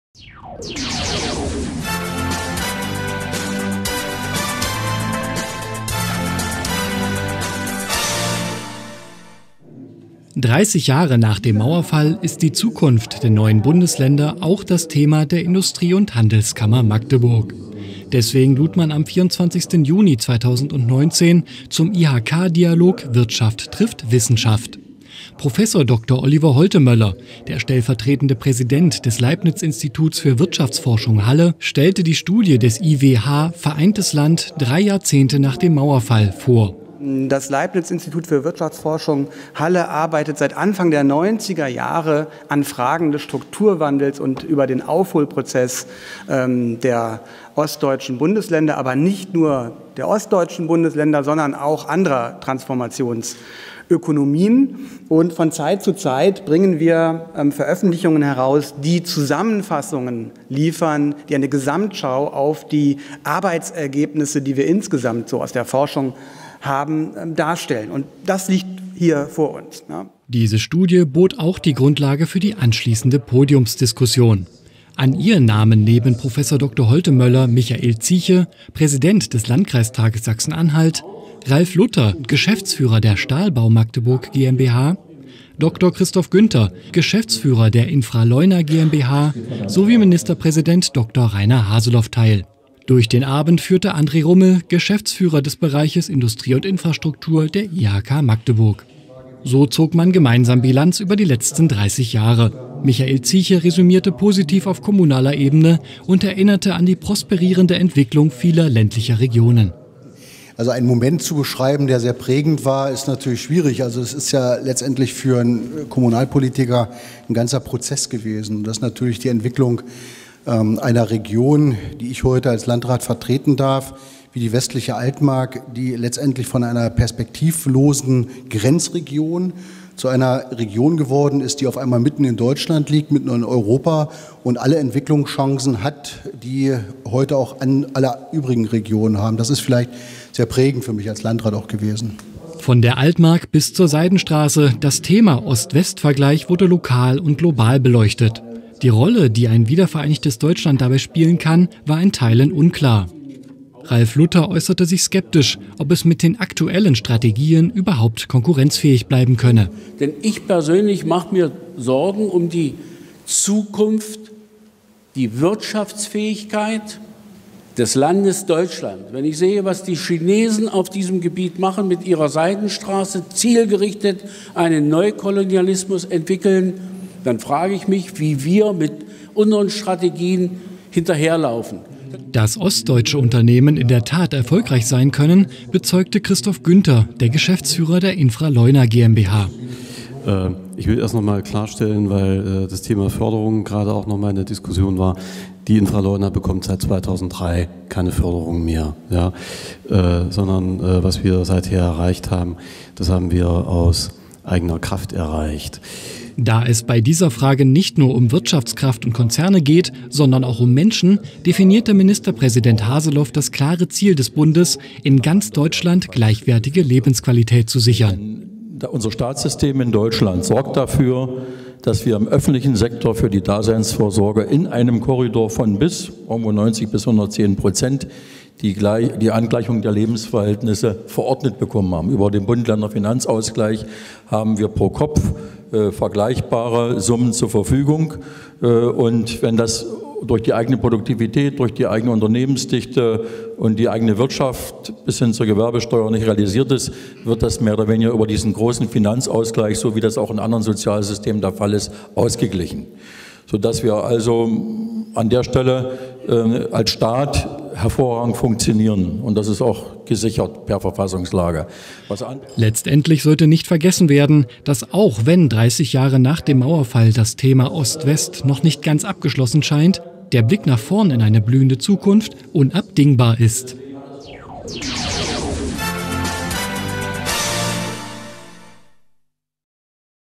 IHK-Dialog Wirtschaft trifft Wissenschaft (24.06.2019)
Wo steht Ostdeutschland 30 Jahre nach dem Mauerfall? Das war das Thema einer Diskussionsveranstaltung u.a. mit Ministerpräsident Dr. Reiner Haseloff und IWH-Vizepräsident Prof. Dr. Oliver Holtemöller in der IHK Magdeburg.